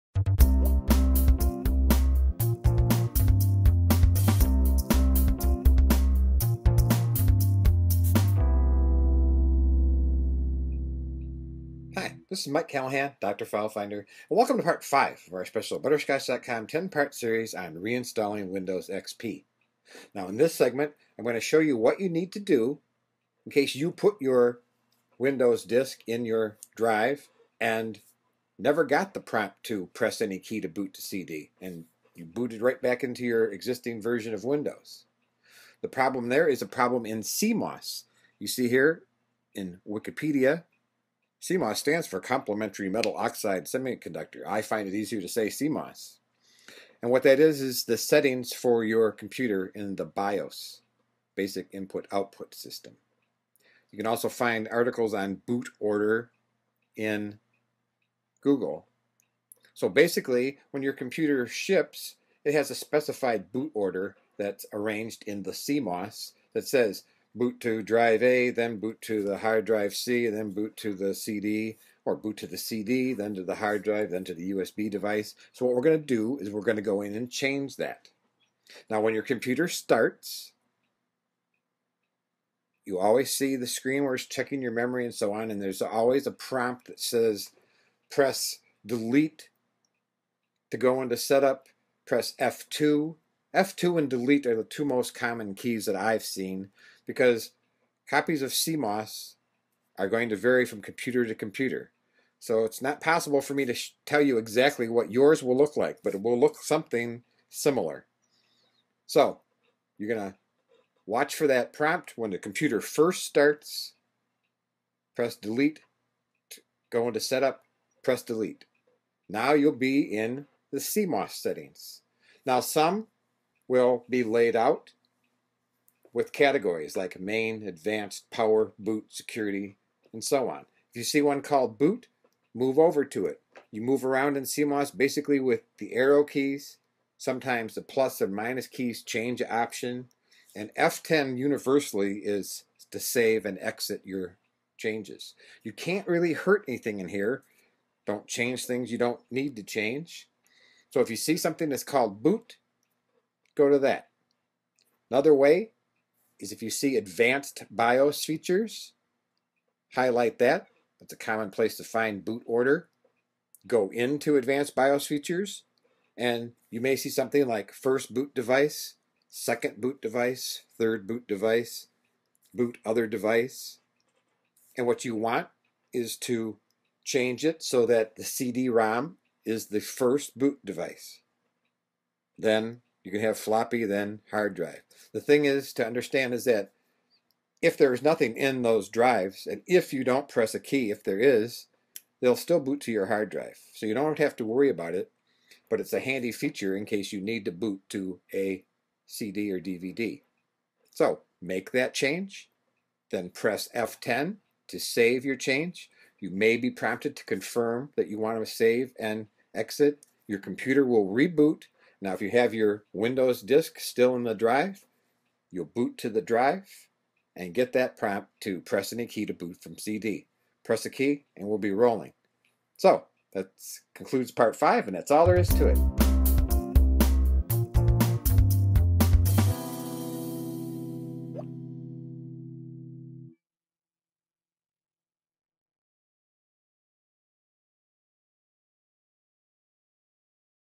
Changing the Windows XP boot order
Sometimes when you put your CD in the drive and restart Windows it just reboots and goes straight back to Windows. Misses your CD all together.  A Internet tutorial by butterscotch.com.